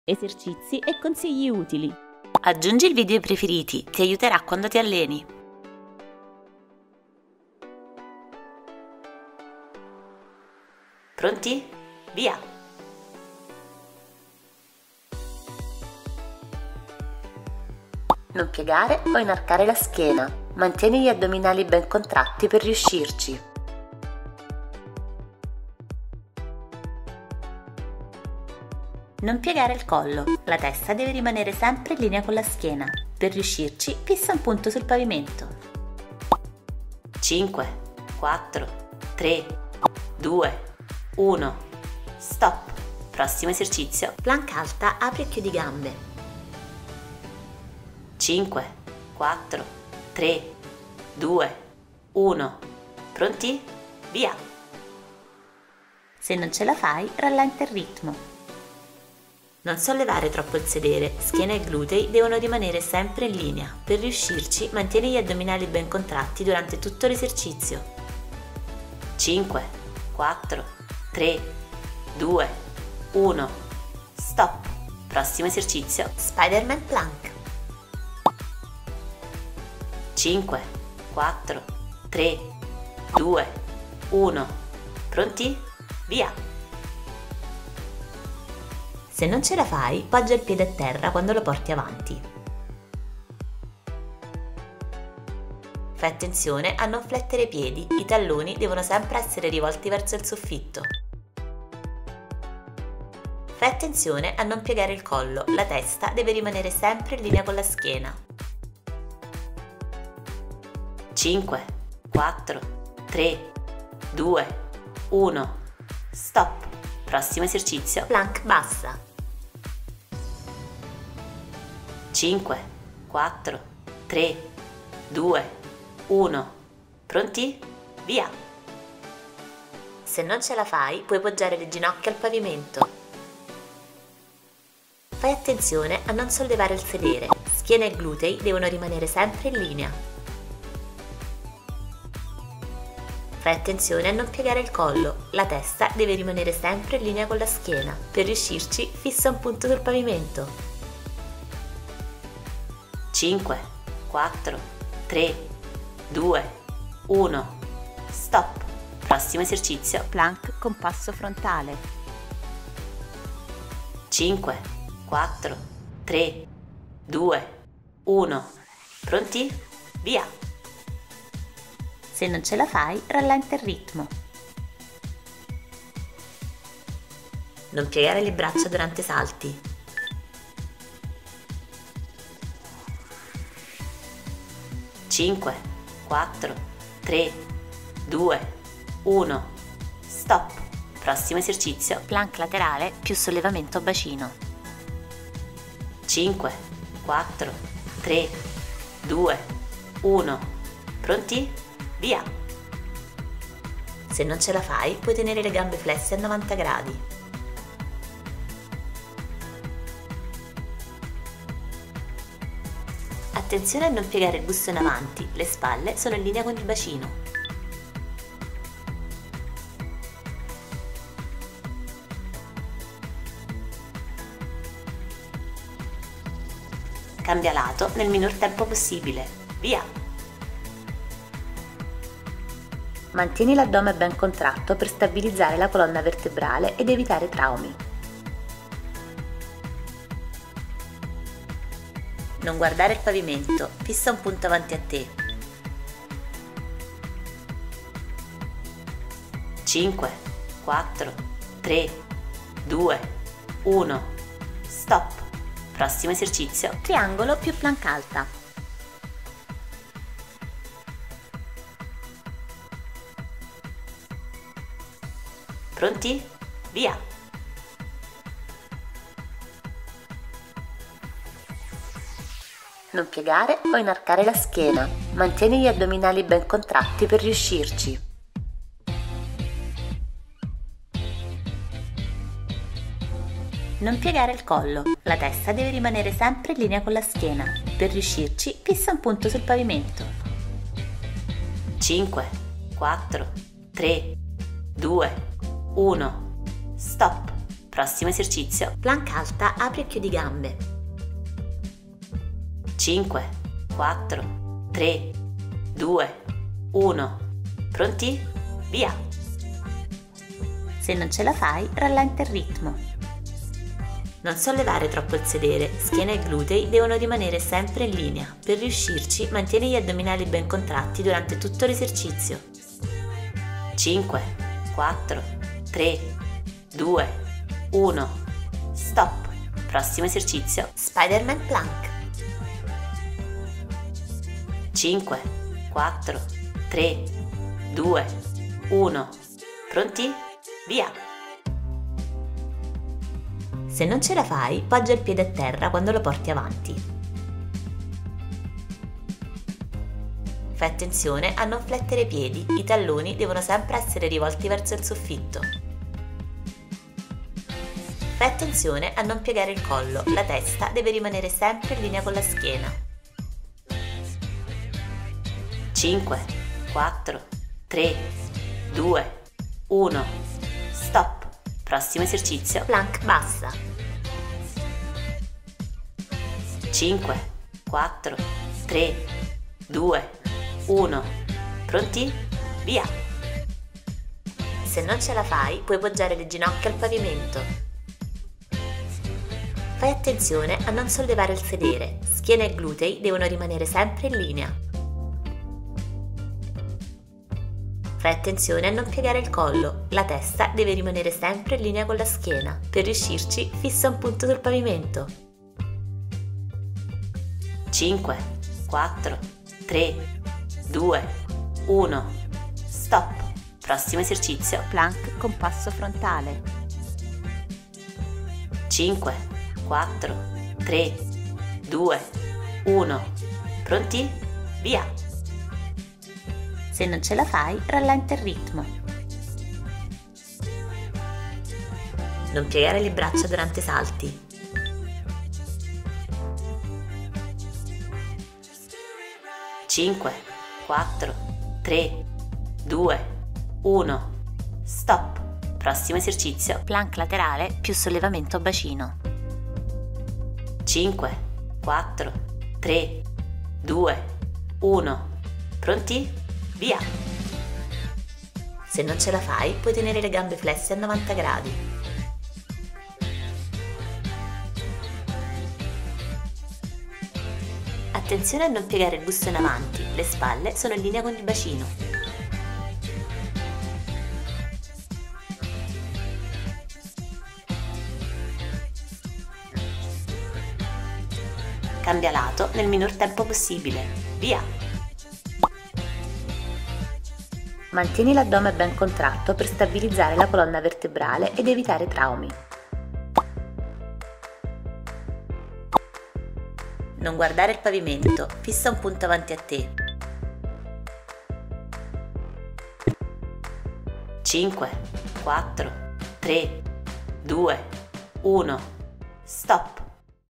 Addominali Perfetti in 8 Minuti
Esercizi per avere addominali perfetti in 8 minuti di allenamento quotidiano. Esercizi mirati per avere una pancia piatta e tonica.

⦿ Calorie Bruciate: 40 - 70

⦿ Frequenza di allenamento: ripeti il workout 5 volte a settimana

❤ - Altri video di Fitness?

Consulta il medico prima di fare questi esercizi.

Benessere360 - Lumowell - W385